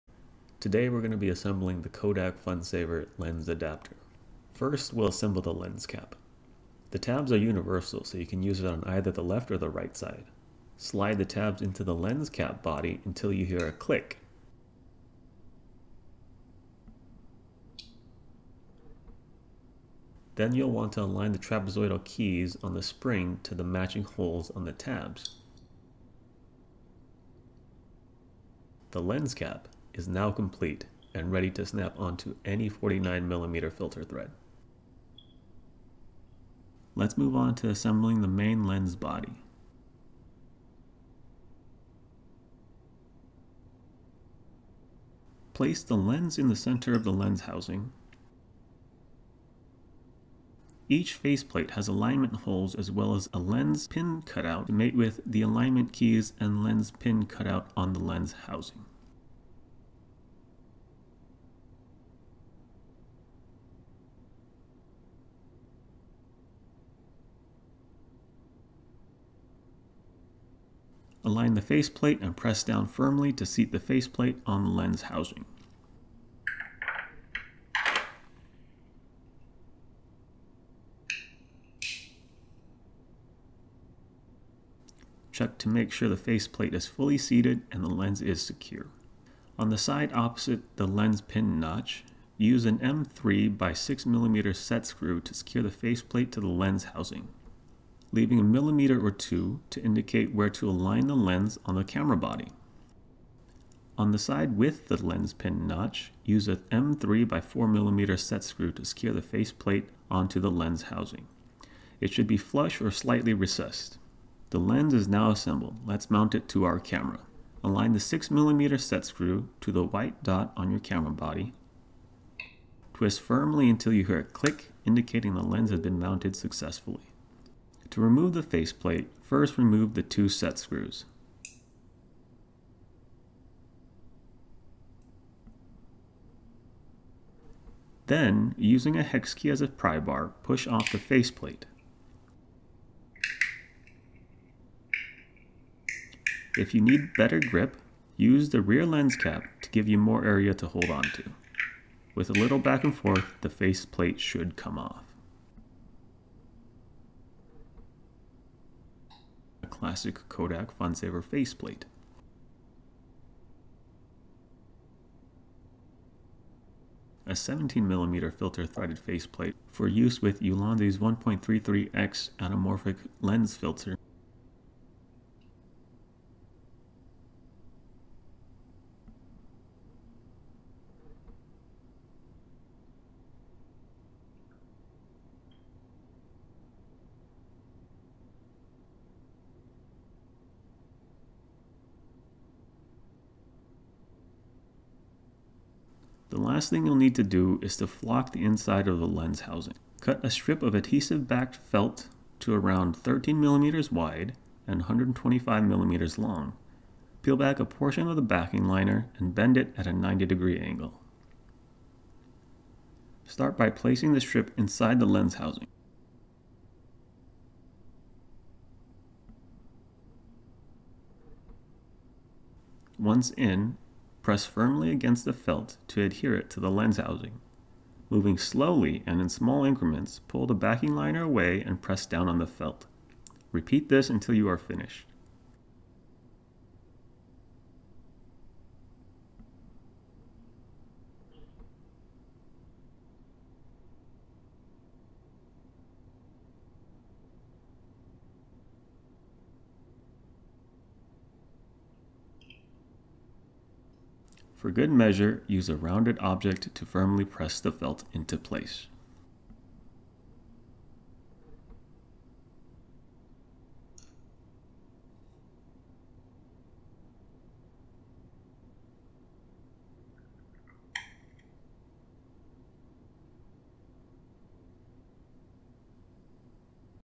3D Printed 30mm Kodak FunSaver Lens Adapter Assembly
I'm going to show you how to assemble your 3D printed Kodak FunSaver Lens Adapter.

What you'll need:

1x M3 x 6mm set screw

1x M3 x 4mm set screw

1x 1.5mm hex key/allen wrench

1x 13mm x 125mm adhesive backed felt

3d printed Kodak FunSaver Lens Adapter

EDITOR'S NOTE: The links found at my original store are no longer updated or available and have been migrated to github. For posterity, the links are included here:
Lens Adapter STLs or Pre-production 3D printed lenses available for purchase